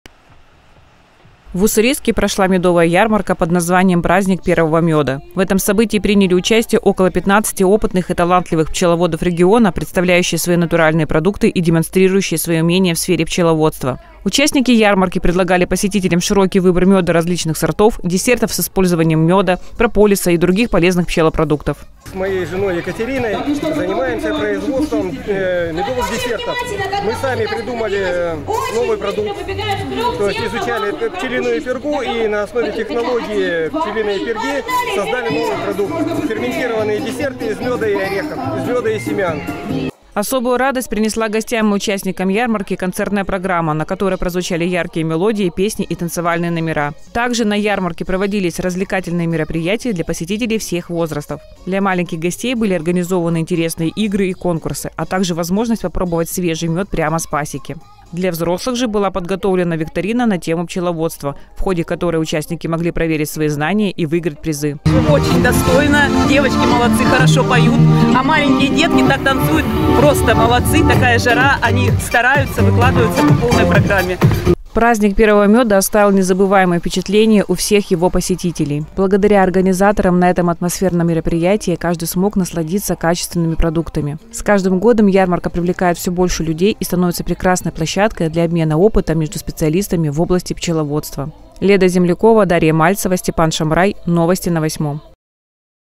«Праздник первого мёда»
В Уссурийске прошла медовая ярмарка под названием «Праздник первого мёда». В этом событии приняли участие около 15 самых опытных и талантливых пчеловодов региона, представляющих свои натуральные продукты и демонстрирующих свои умения в сфере пчеловодства.